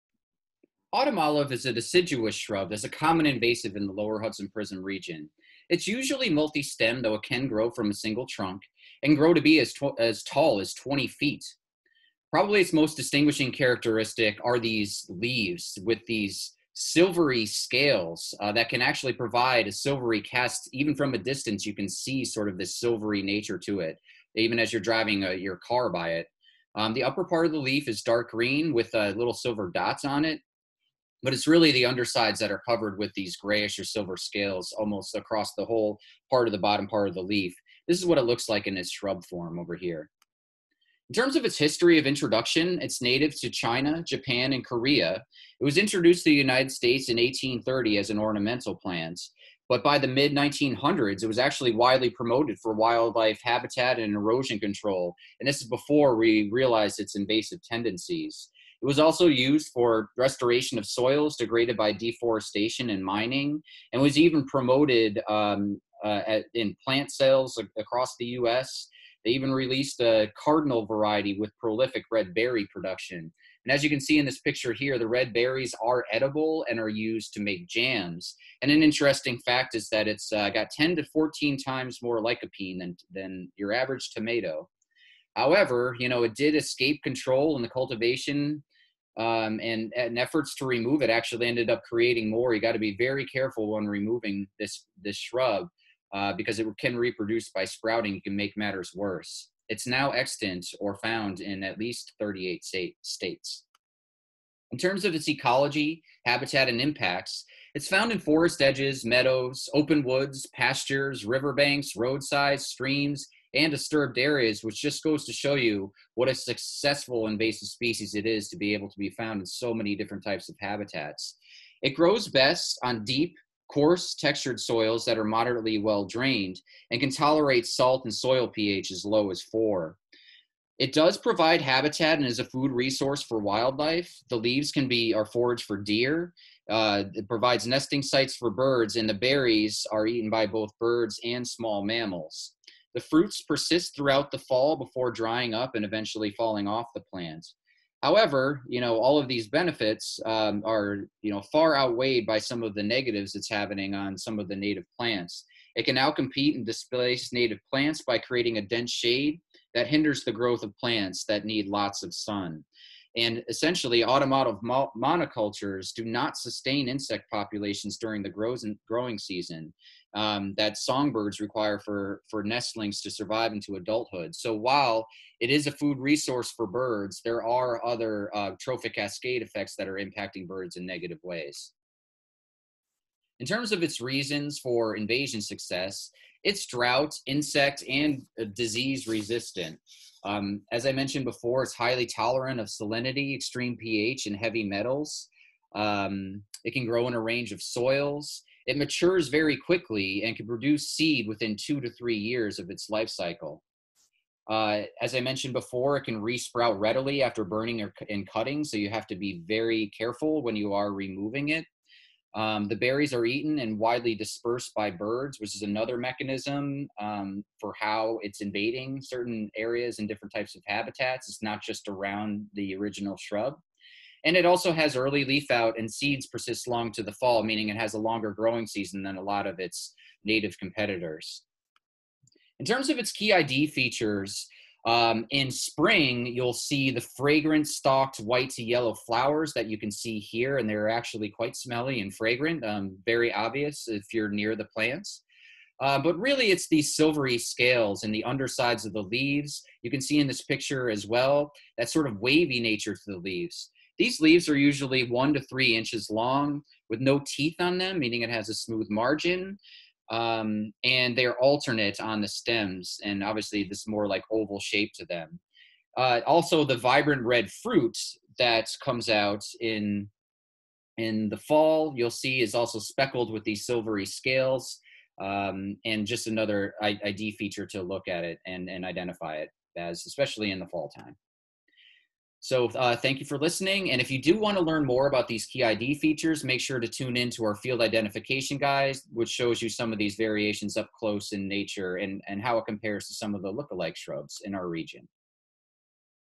Autumn Olive Species Spotlight
This video describes the introduction history, ecology and impacts of autumn olive, a common invasive deciduous shrub in the Lower Hudson PRISM region. It also describes autumn olive's key ID features and links to a more detailed field ID video.